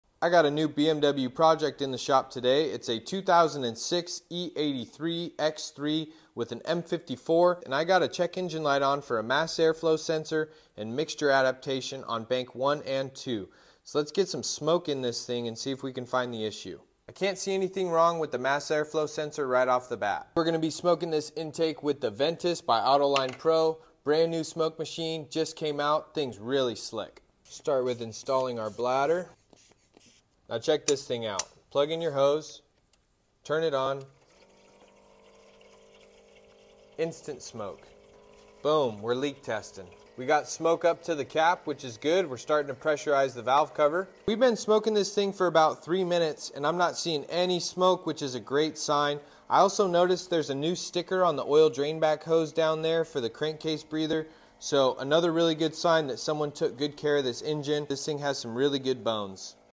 How to Diagnose Check Engine Mass Airflow Sensor M52 | Smoke Test Vacuum Leak | AutoLine Pro
Discover a seamless solution to diagnosing automotive leaks with Autoline Pro's new Ventus Smoke Machine. In this video, join us as we tackle a BMW project, putting the Ventus to the test. Get an insider look as we work with the brand-new machine, instant smoke formation, and an efficient leak-testing process. Watch as we illuminate the state-of-the-art features of the Ventus Smoke Machine that make it a valuable addition to any automotive workshop. This tool not only simplifies auto diagnostics, but also ensures your engines are maintained at their best.

for more on our innovative product lineup.

Follow us for the latest updates and expertise @autolinepro.